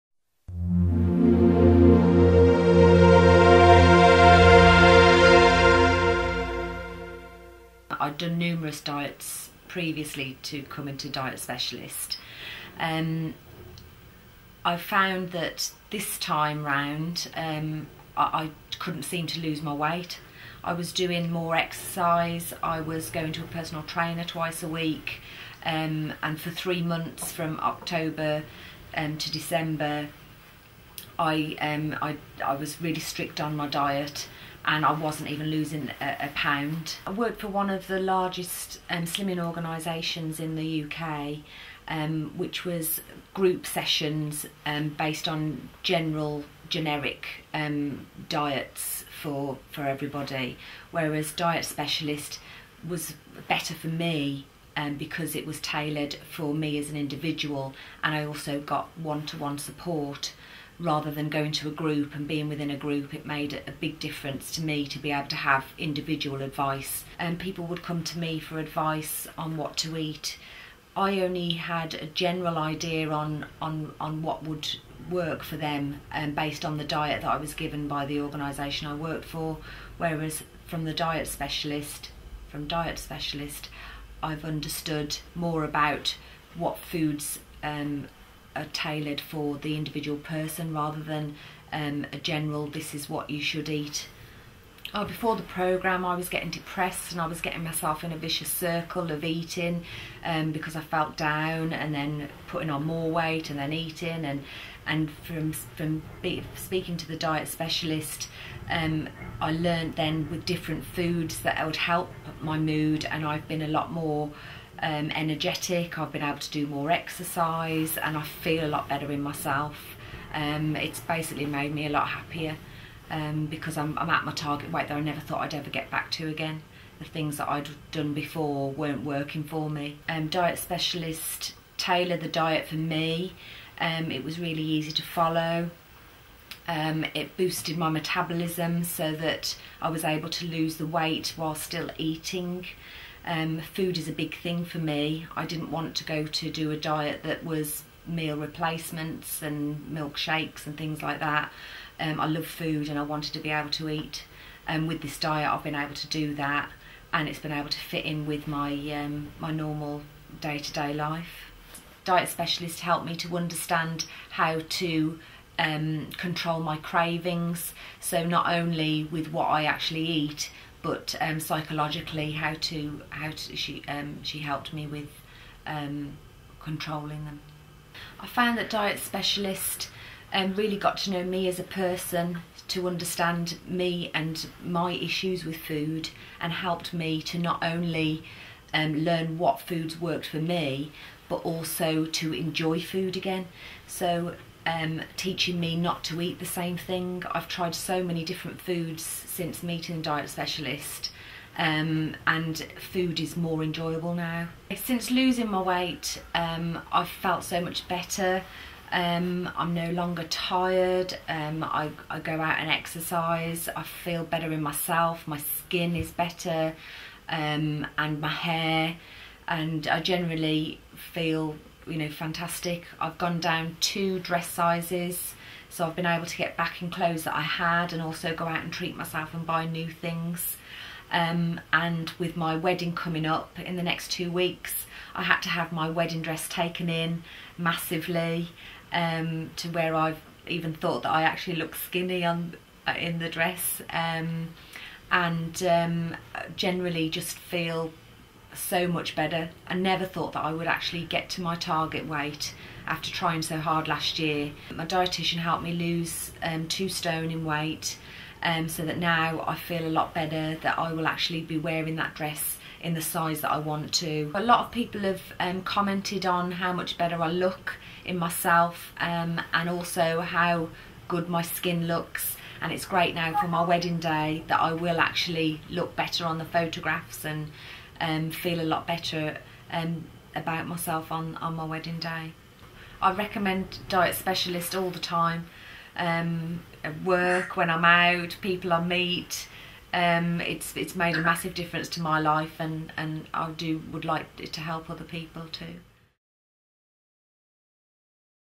Former Slimming consultant loses 2 stones with Diet Specialist
A former Slimming consultant at one of the UK's leading slimming companies, losing weight should have been easy. However when it came to her own weight loss for her wedding, the last few stones, were impossible to move. Learn how help her achieve her goal